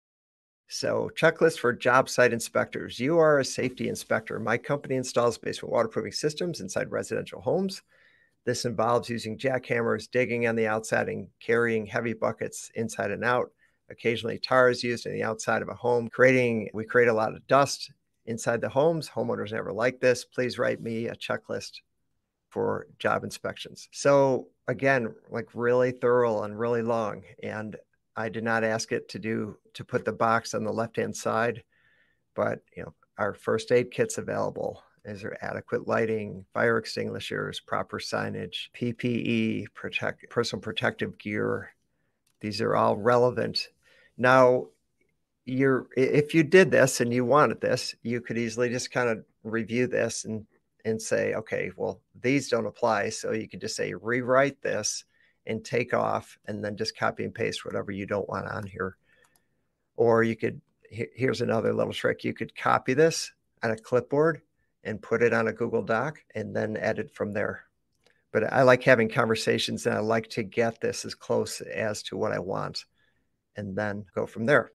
2024-09-26
Checklists made simple with ChatGPT! ✅✍️

Discover how to create efficient and organized checklists in seconds, streamlining your tasks like never before!